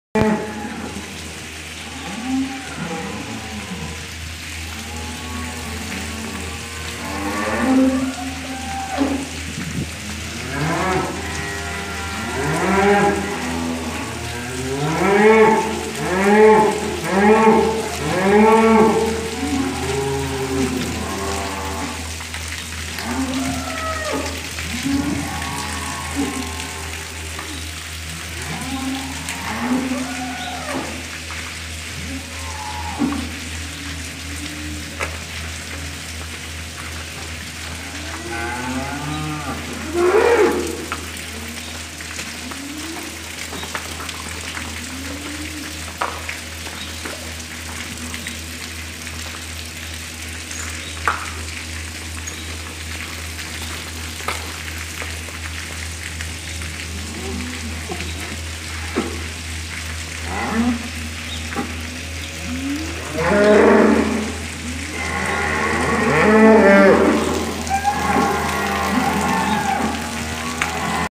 Crisis: Tambero entrerriano aparta las vacas lecheras para mandar a faena
Las imágenes del diario La Nación muestran un momento crítico para un tambero de Gualeguay, Entre Ríos, cuando selecciona las vacas lecheras de las que se desprenderá por la crisis que atraviesa el sector